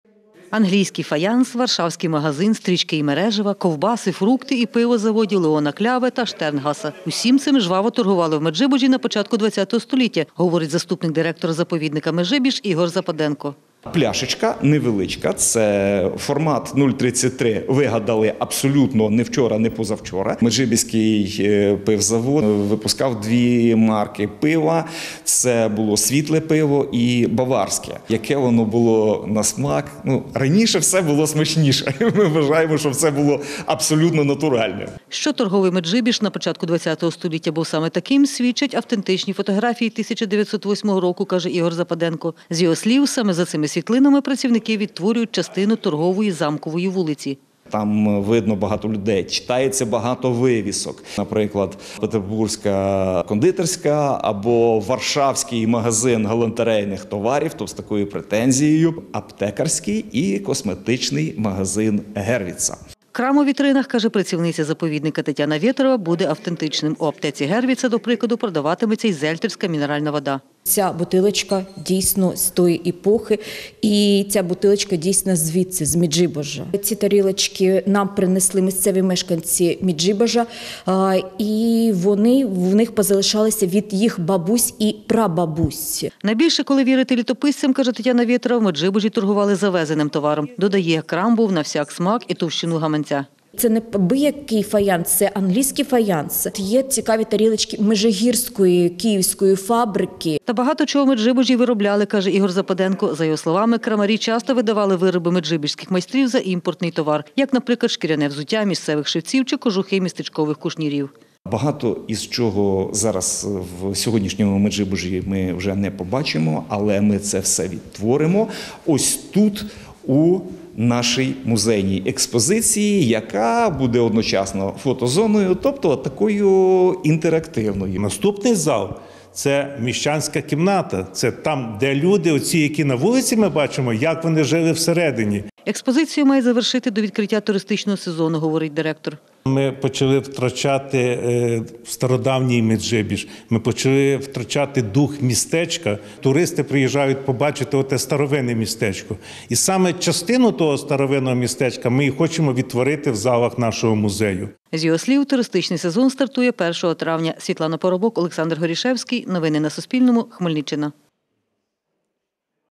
Яким був торговий Меджибіж на початку 20-го століття, можна буде побачити в державному заповіднику
Яким був торговий Меджибіж на початку 20-го століття, можна буде побачити в державному заповіднику: там до відкриття туристичного сезону готують експозицію «Меджибіж торговий»-розповідає заступник директора заповідника Ігор Западенко.  За його словами, працівники відтворюють частину центральної, Замкової, вулиці за фотографіями 1908-го року.